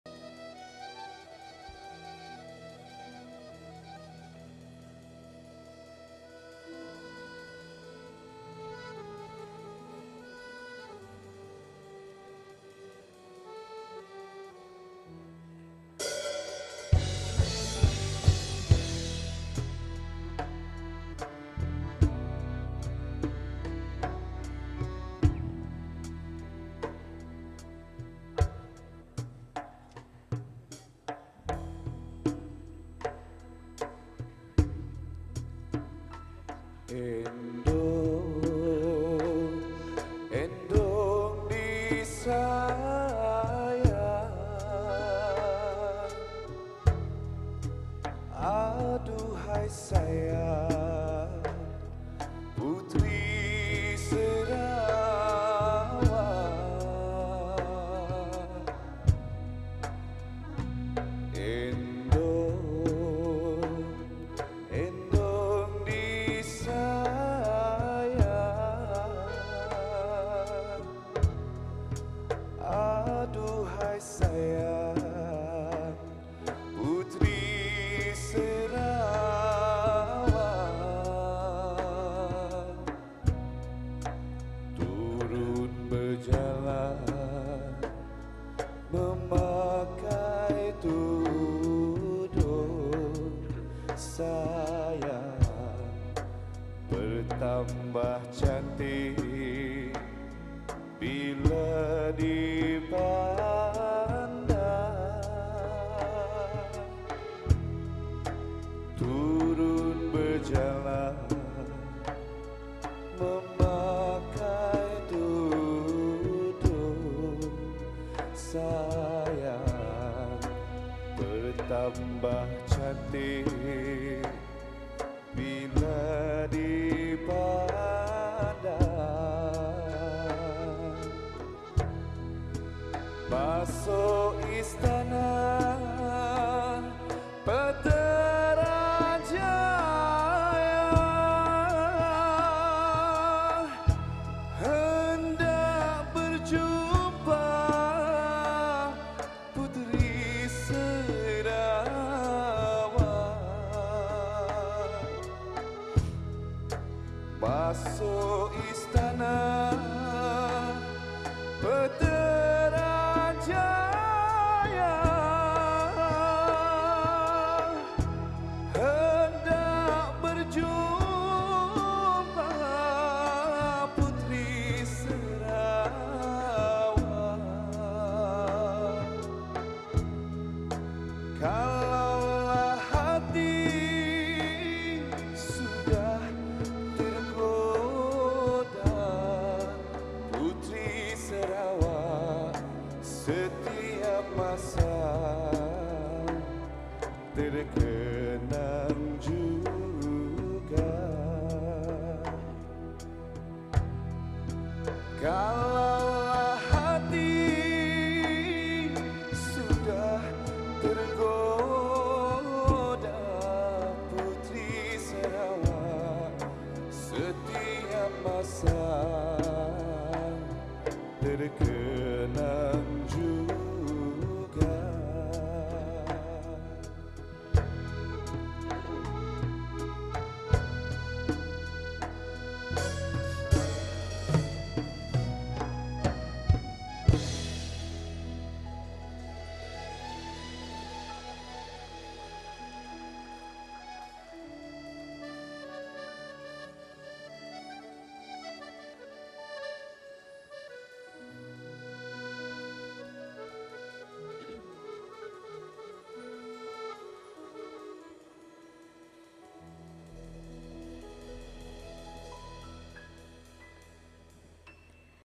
Tarian Putri Sarawak | Global Muslim Forum
Tarian 'Putri Sarawak' telah dipersembahkan pada Makan Malam Gala Global Muslim Business Forum 2023 di Borneo Convention Centre, Kuching semalam.